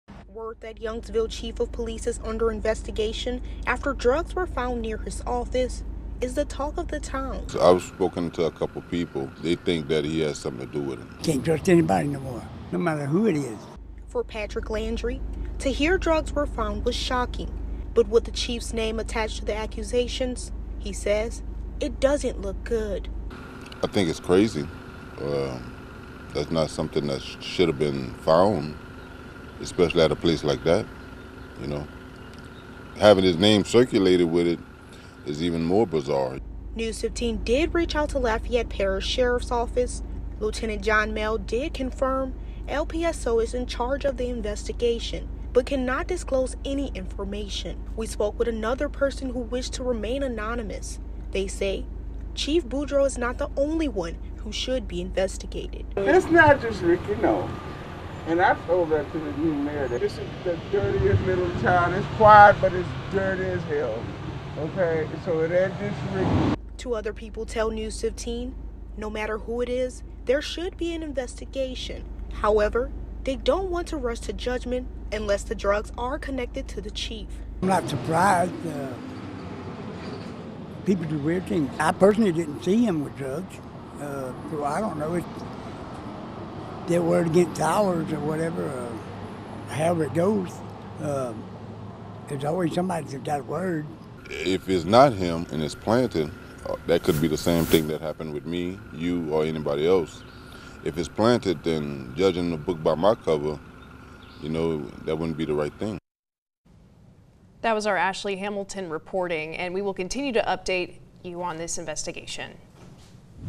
Youngsville residents react to police chief drug use probe
-Word that Youngsville's Chief of Police is under investigation after drugs were found near his office is the talk of the town.